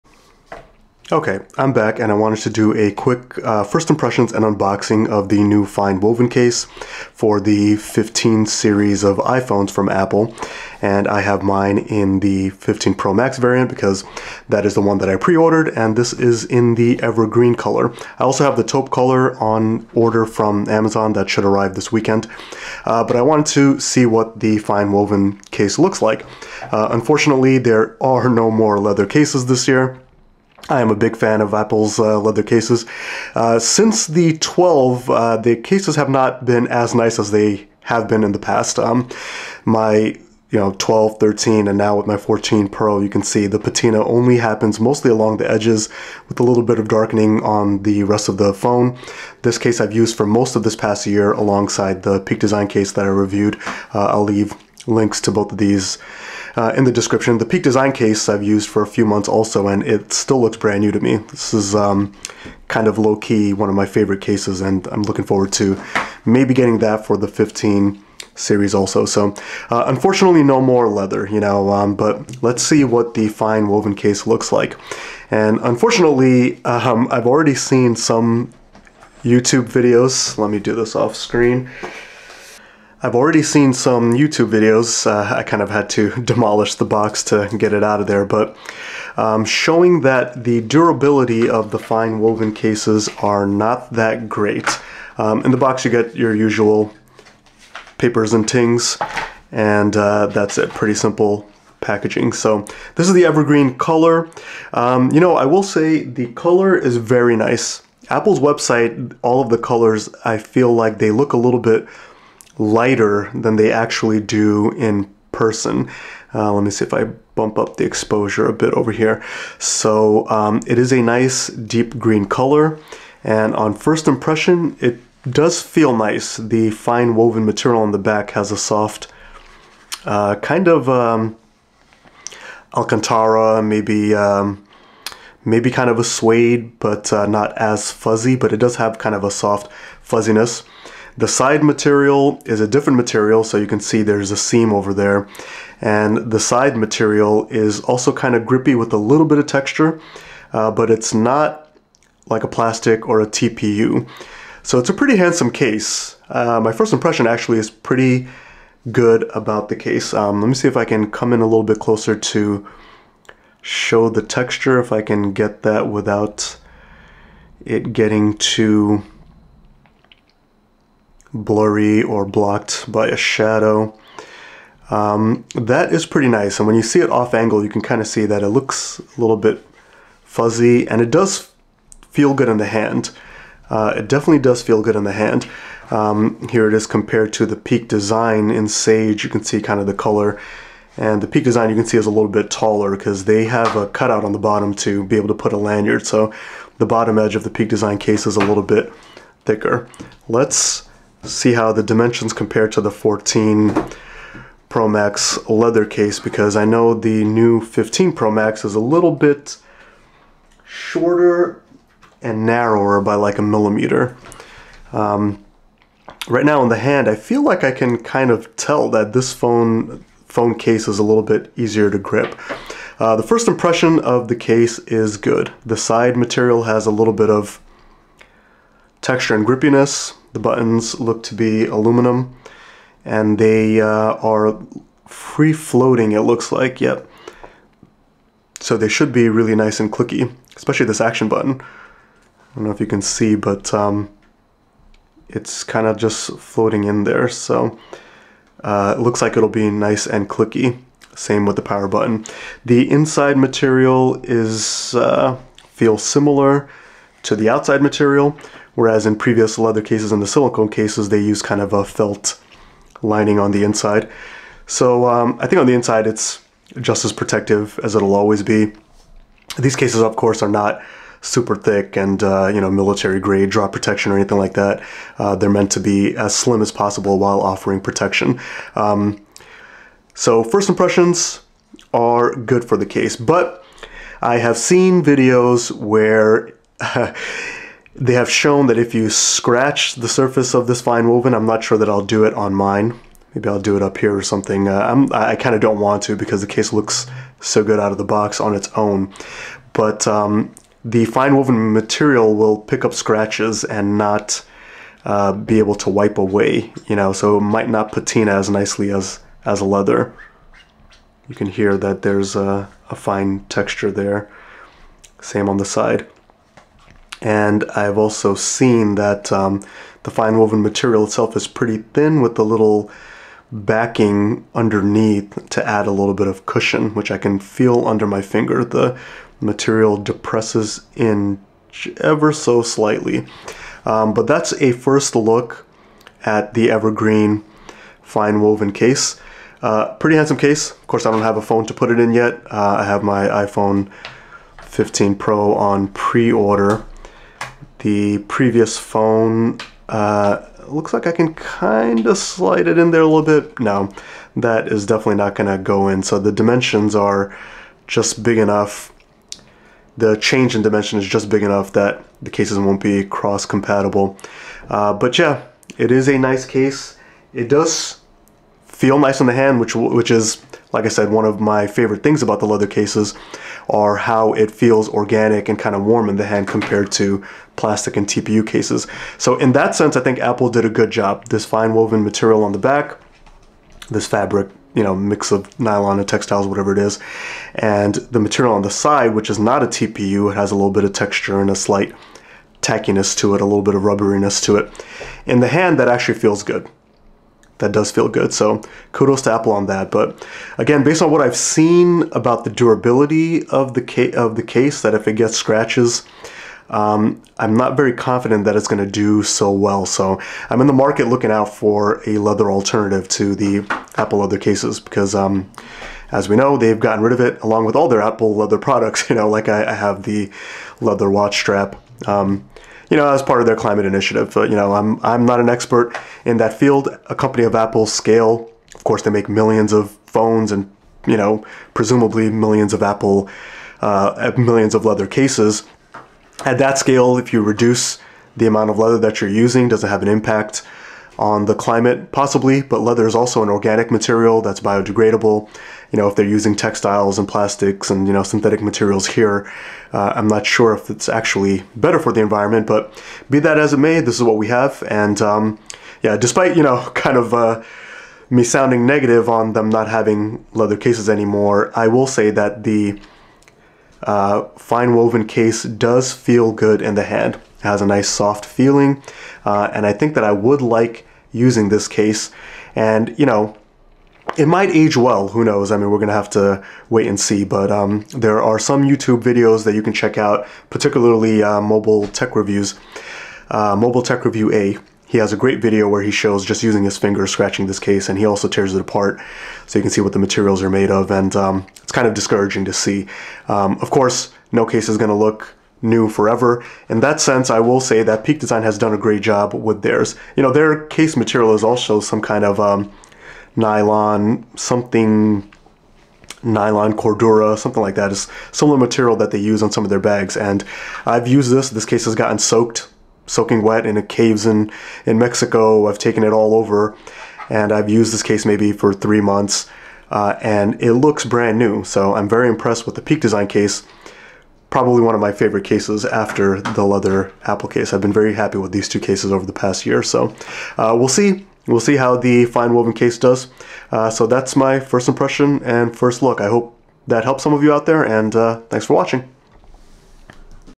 Apple FineWoven Case Unboxing and First Impressions
First look at the new evergreen fine-woven case from Apple for the iPhone 15 pro max.  A handsome case, and good first impression, but definitely sad about seeing the leather cases discontinued!

Apple iPhone 15 pro FineWoven
Peak Design Case